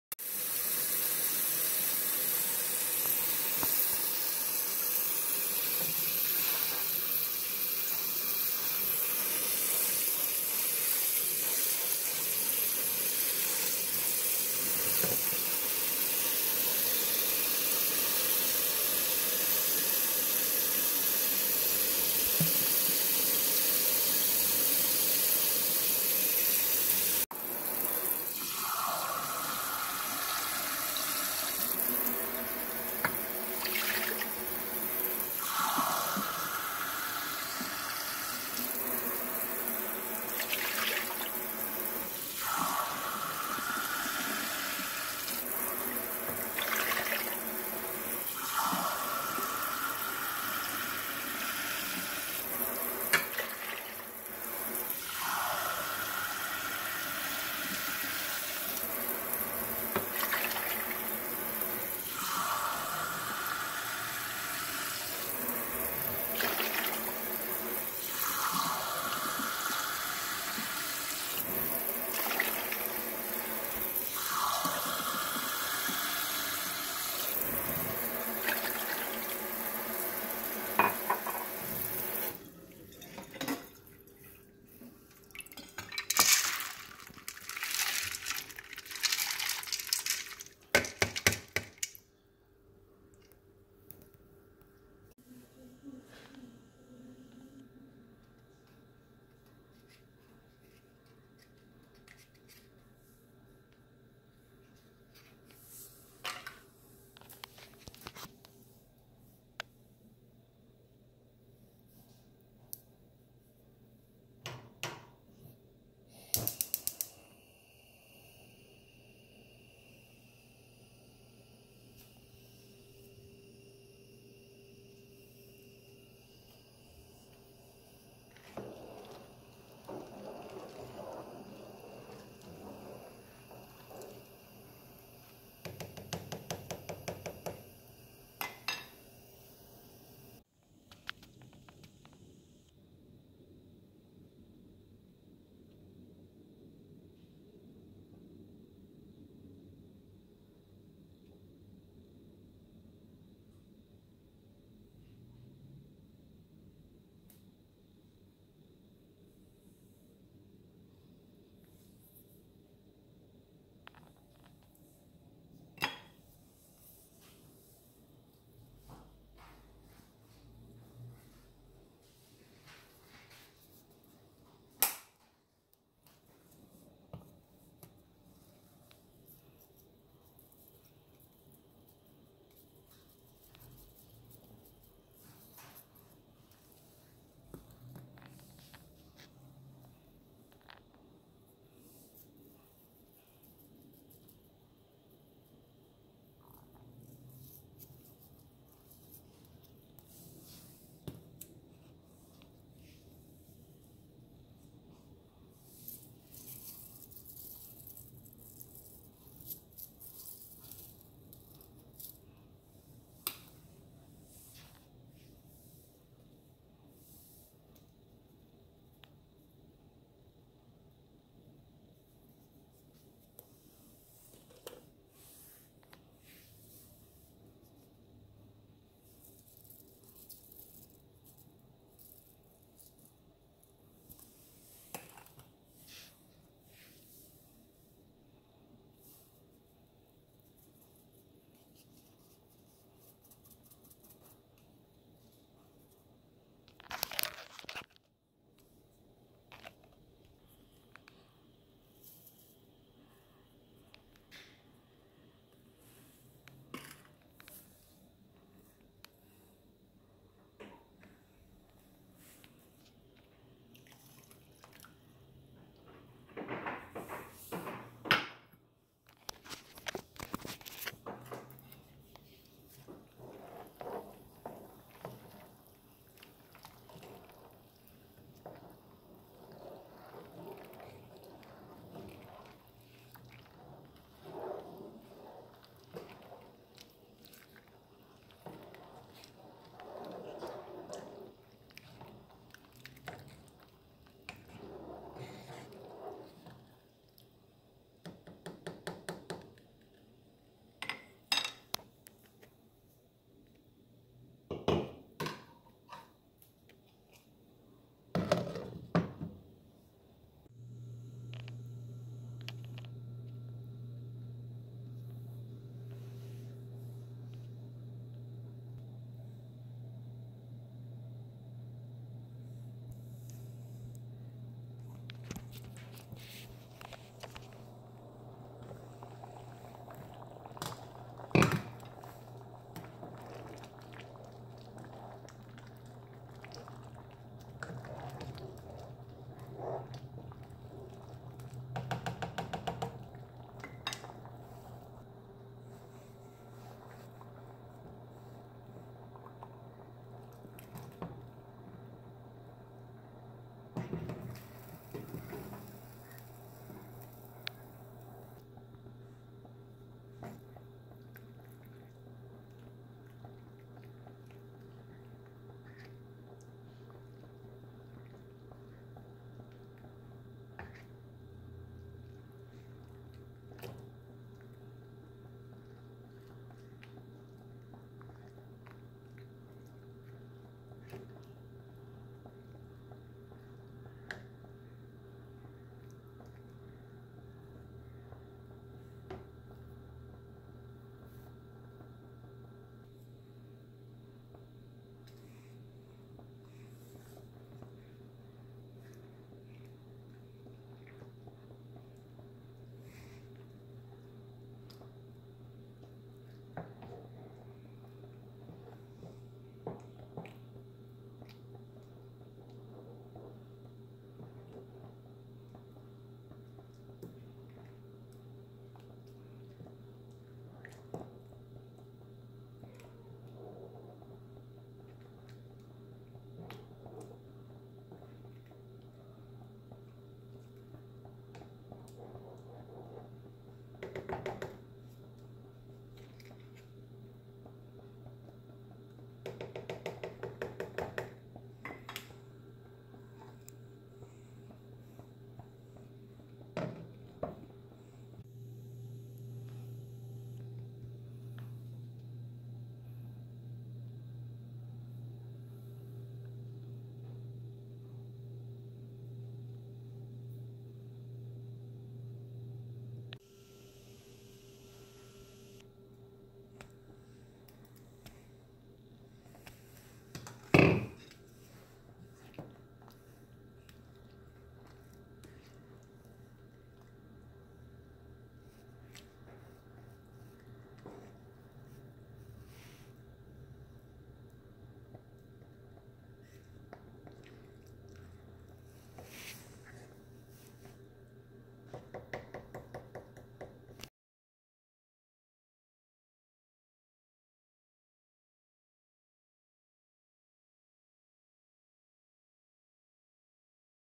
Homemade 15 Bean Soup with Sausage! ❤
Sorry no music and voice overs, but enjoy and try it! 😘